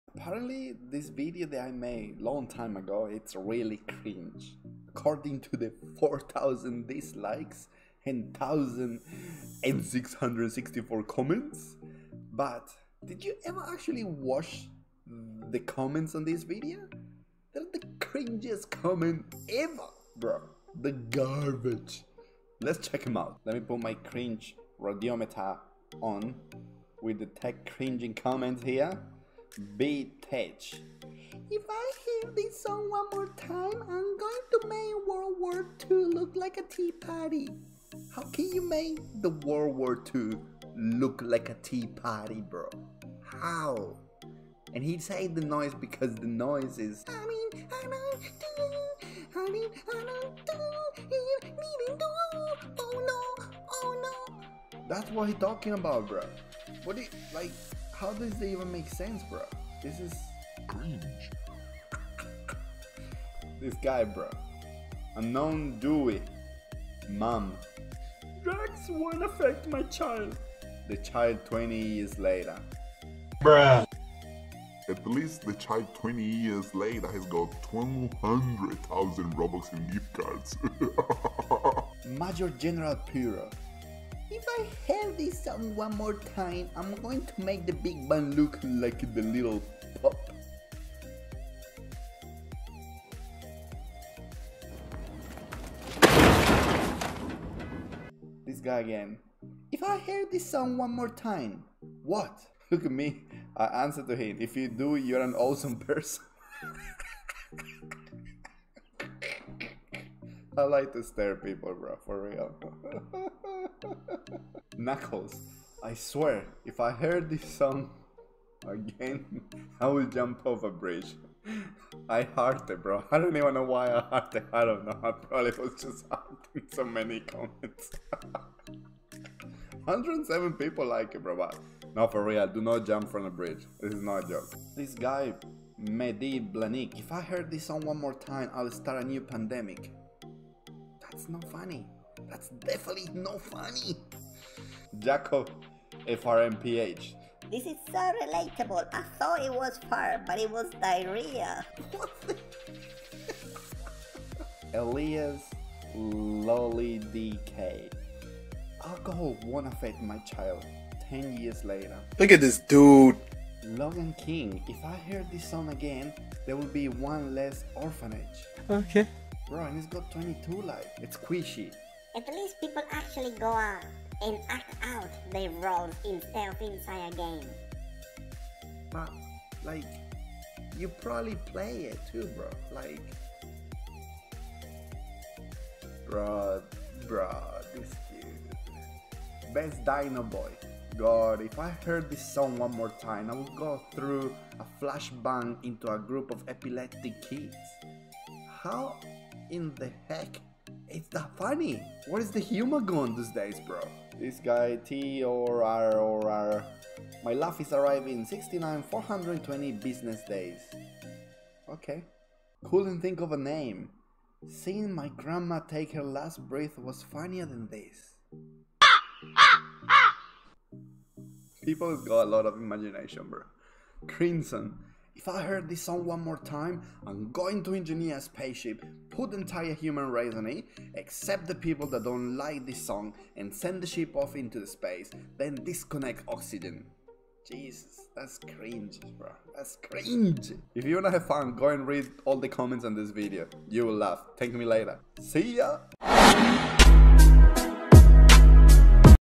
👽 CRINGEST Comments On My CRINGE Video 🥇
Those are some of The CRINGEST comments i have read in my videos!!
obviously this video is made for fun and i do respect everyone's opinion!

Adioss & Byee